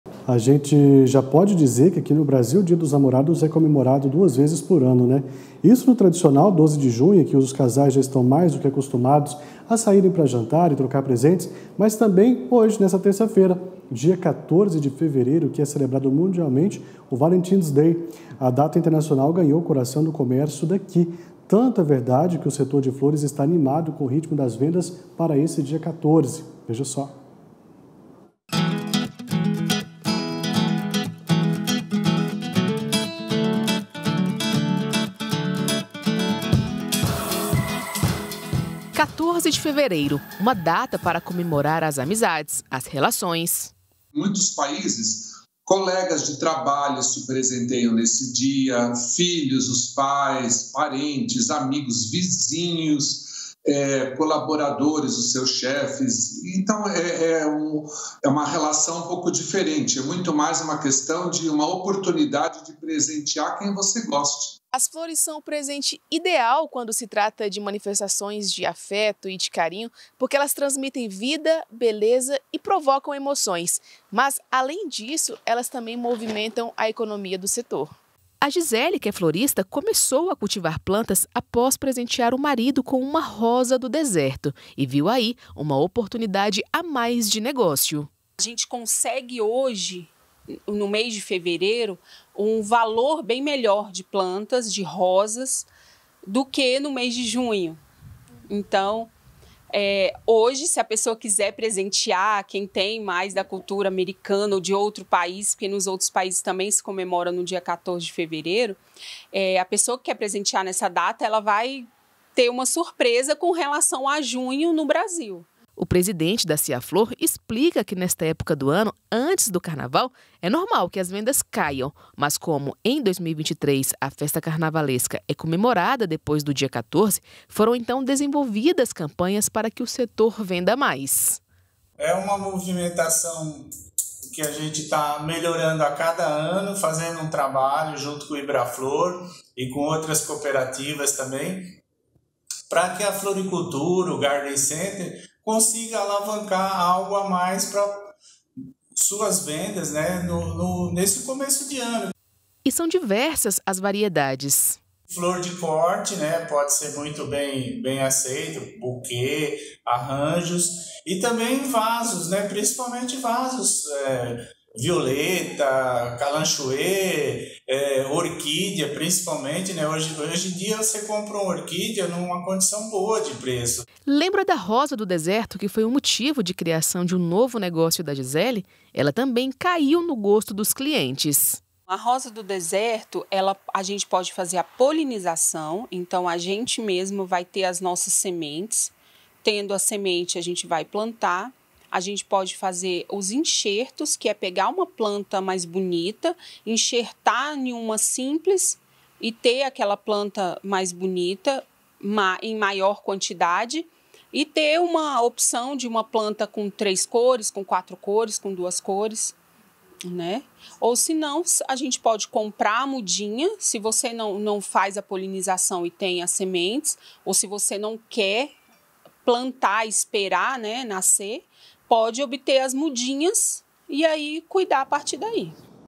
Valentine's day: Data movimenta comércio brasileiro
A gente já pode dizer que, aqui no brasil, o dia dos namorados é comemorado duas vezes por ano. Isso no tradicional 12 de junho, em que os casais já estão mais do que acostumados a saírem pra jantar e trocar presentes, mas também já nesta terça-feira, quando é celebrado mundialmente o popular "Valentine's day". A data internacional 'ganhou o coração' do comércio daqui. Tanto é verdade que o setor de flores está animado com o ritmo das vendas para este dia 14.